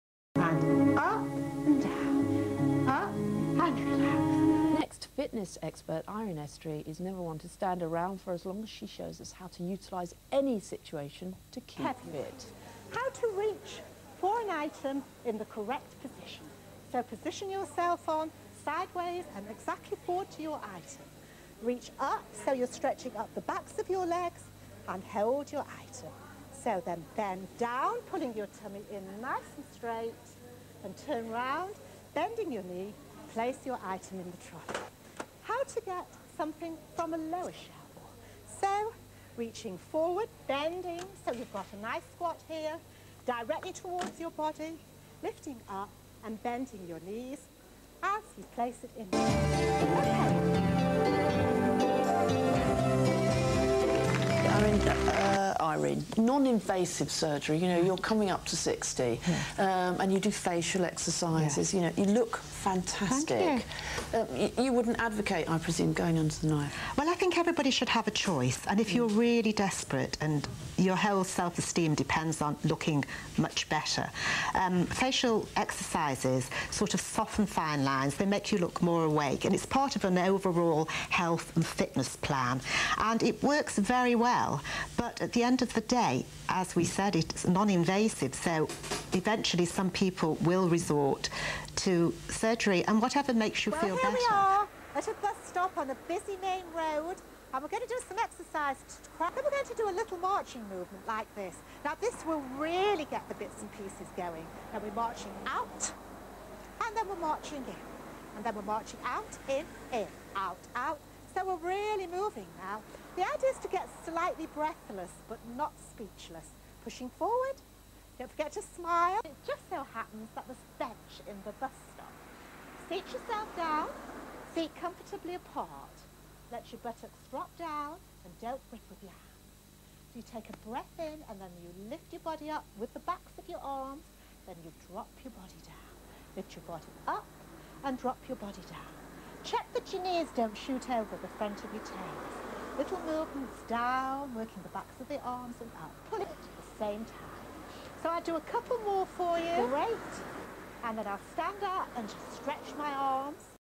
Irene Estry with Caryn Franklin
Irene Estry interviewed by Caryn Franklin and demonstrating exercises in Tesco and at the bus stop.